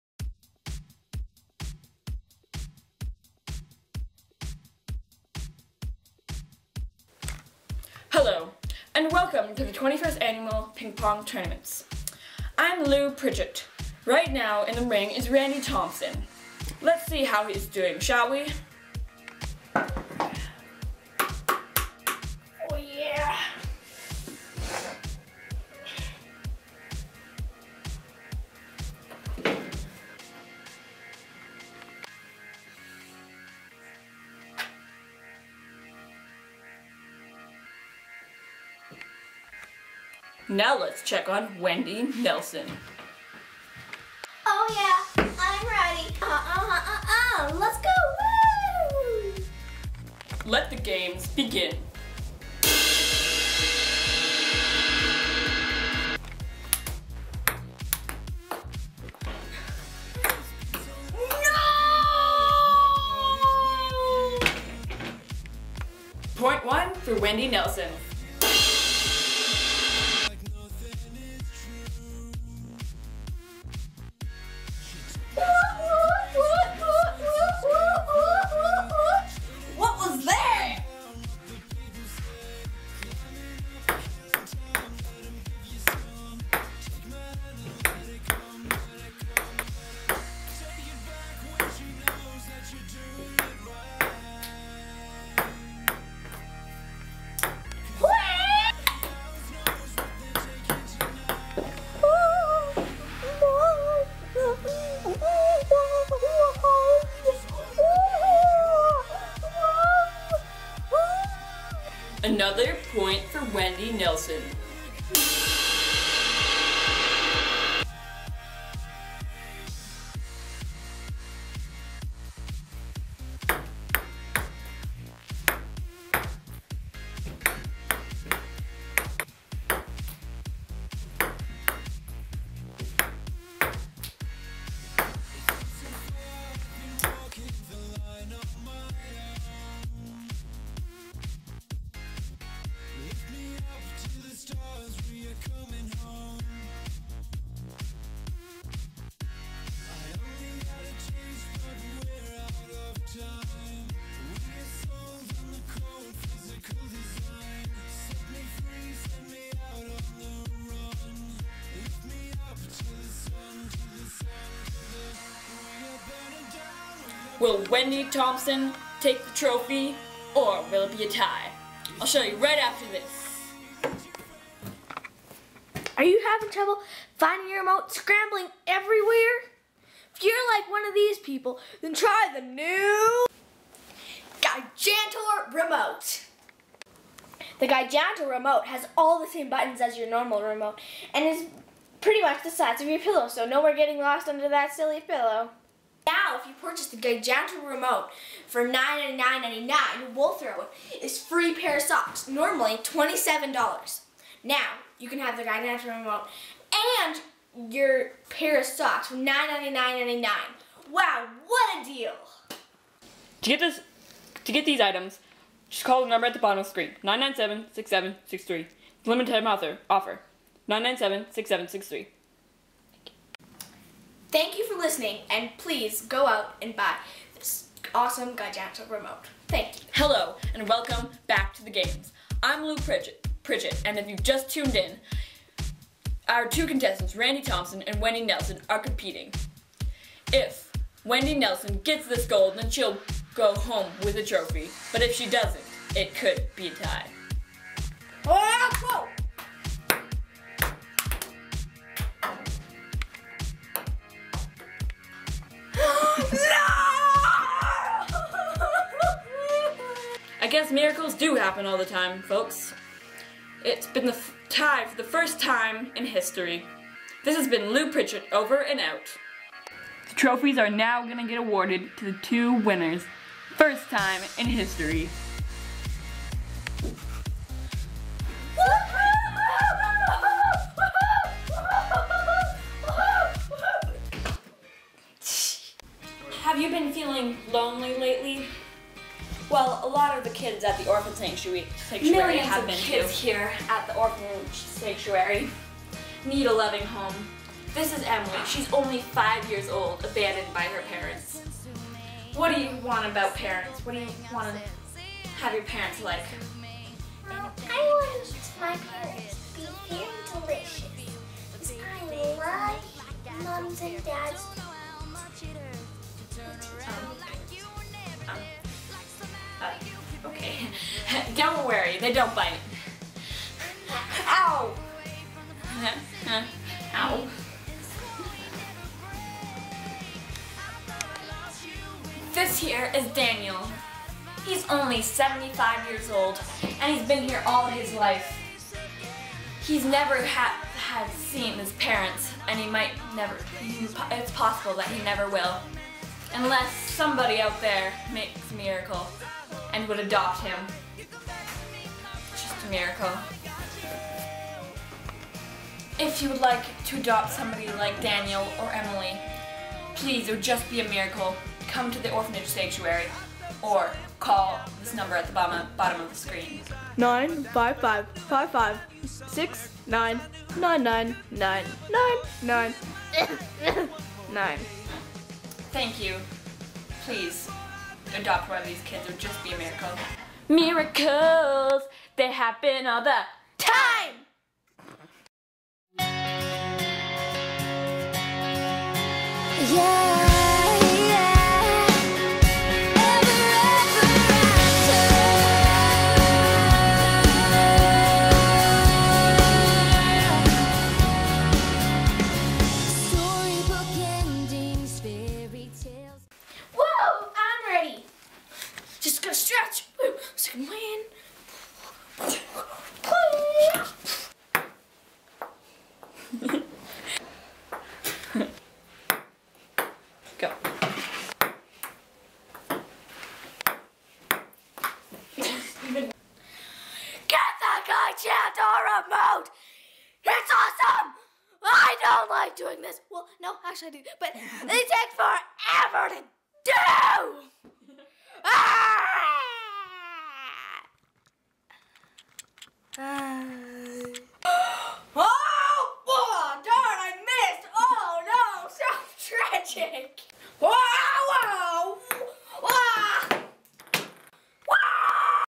Ping Pong Challenges
Randy Thompson vs Wendy Nelson, exciting match.